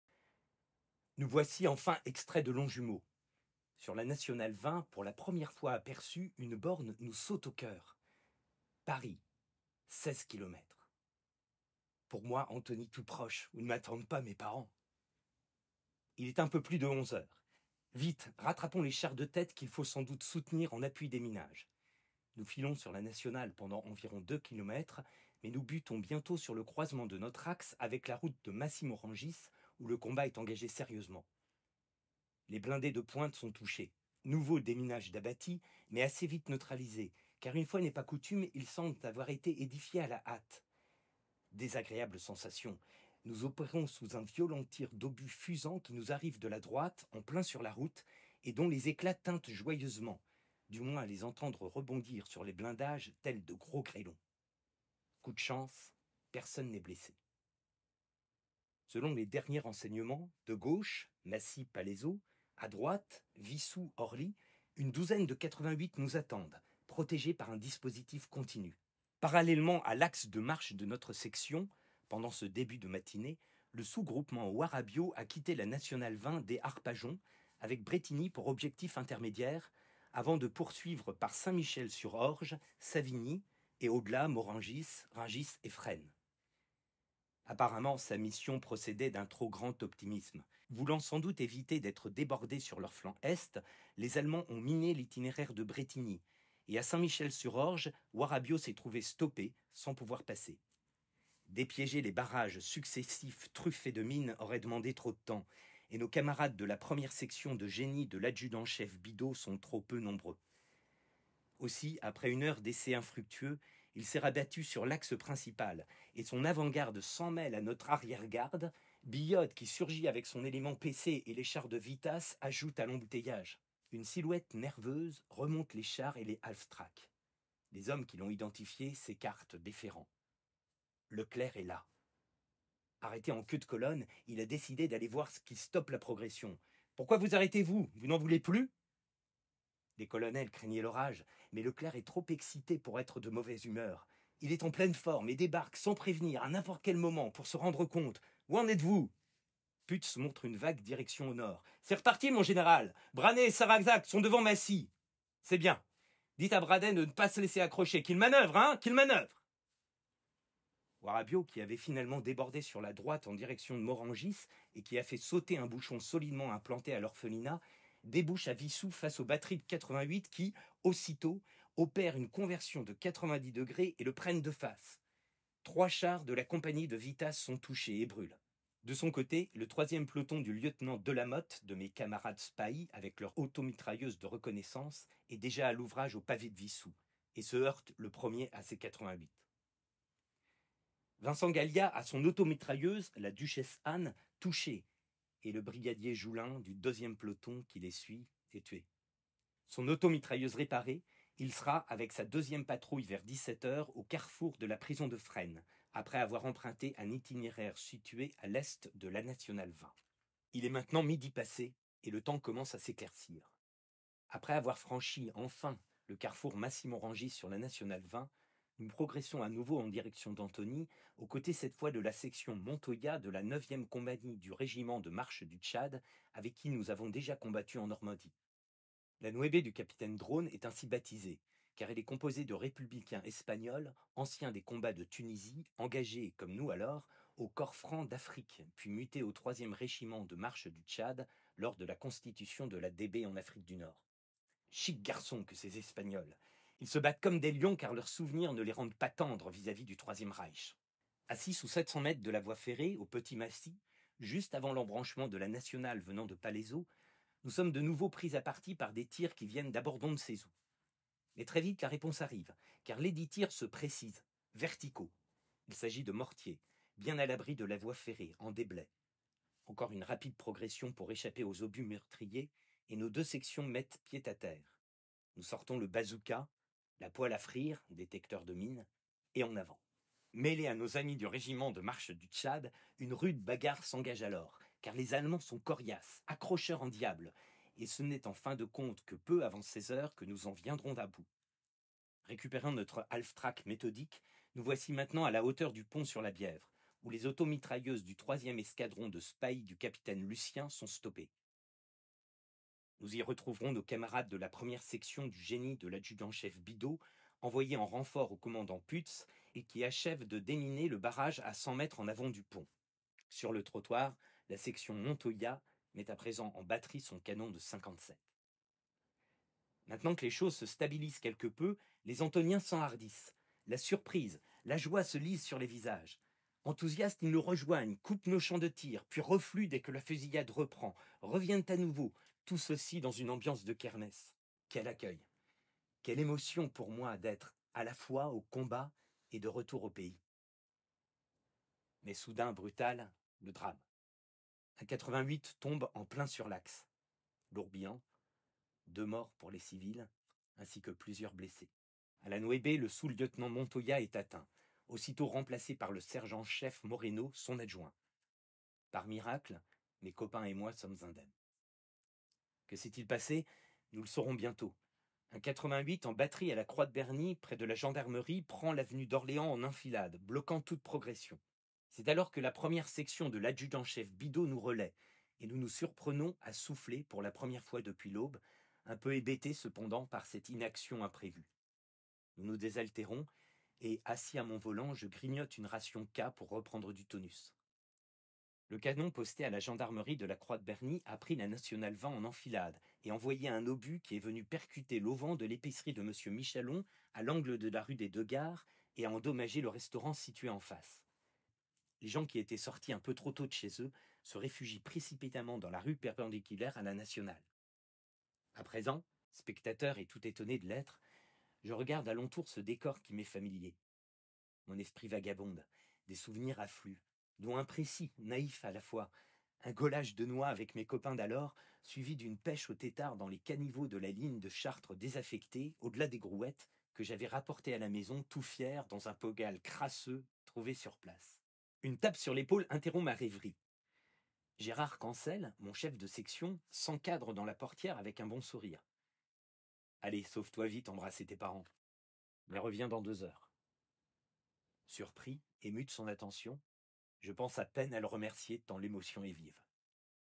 80e anniversaire de la libération d'Antony ep3
Libération d’Antony : témoignage de René Devaux, Antonien, soldat de la 2e DB : épisode 1, les premiers combats

Half-track "LE METHODIQUE" en 1944. Son équipage est composé de 13 hommes, sous le commandement du sergent Reissier, chef d'escouade. René Devaux en est le conducteur - Archives familiales Y. Devaux/ C. Veron, reprod. interdite (Contact Archives communales d'Antony).

René Devaux (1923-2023), Antonien, ancien soldat de 1ère classe de la 2e DB, 13e brigade du génie, colonne Dronne, témoigne en 1990 de sa participation à la libération d’Antony dans le bulletin de l’Association pour la promotion du patrimoine d’Antony (APPA) Antony d’hier et d’aujourd’hui.